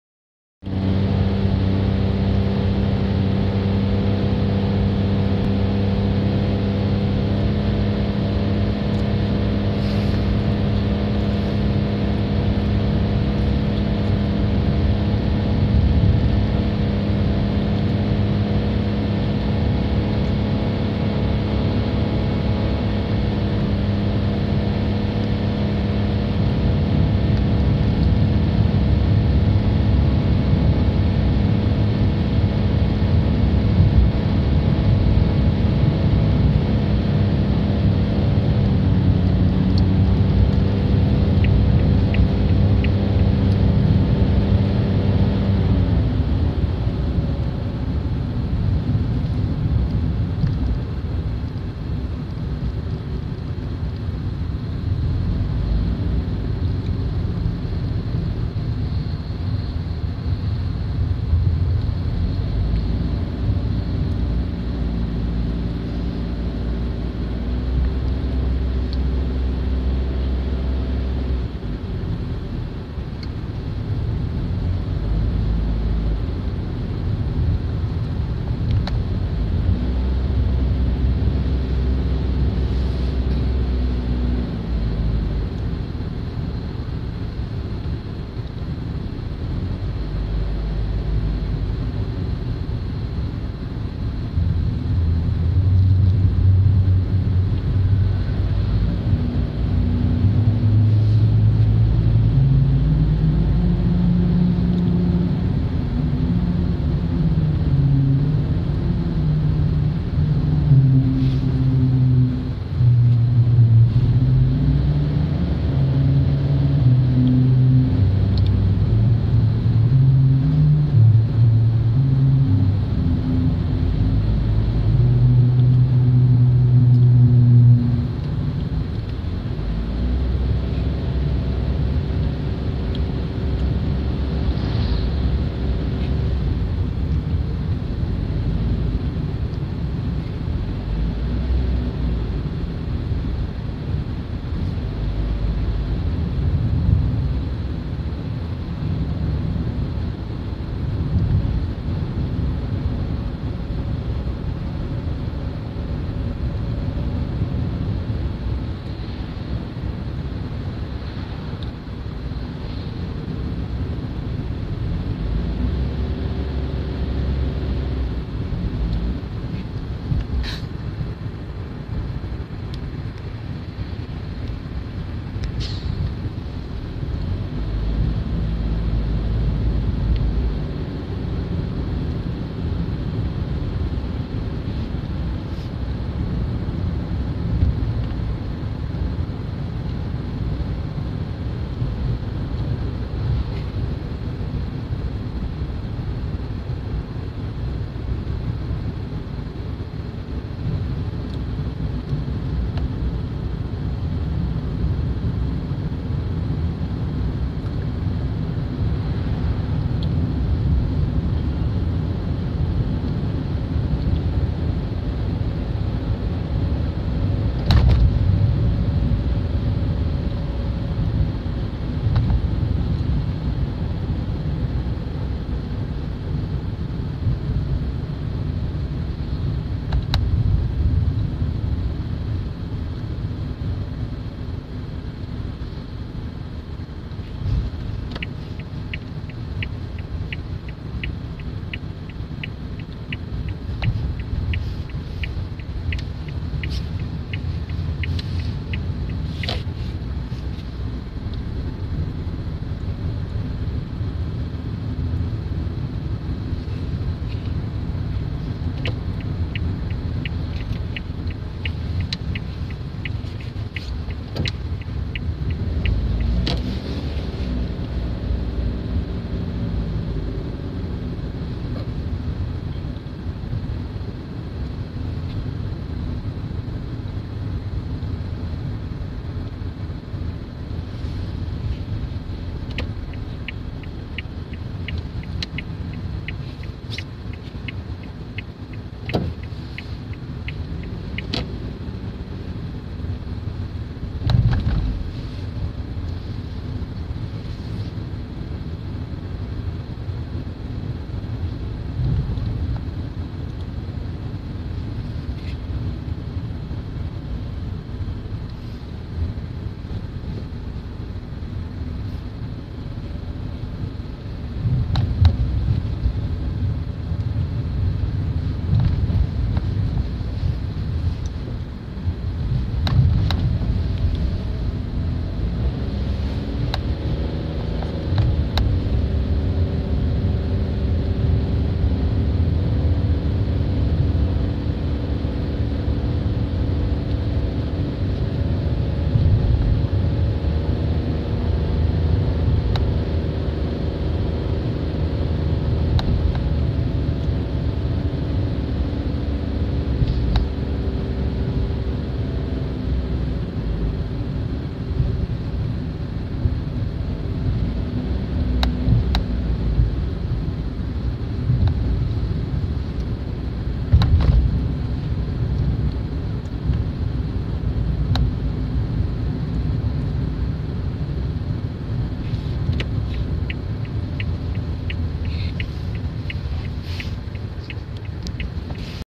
草津温泉 道路状況 2017.11.14 朝７時頃 曇り ５℃ 国道292号線
今日は曇りです。気温は５℃でした。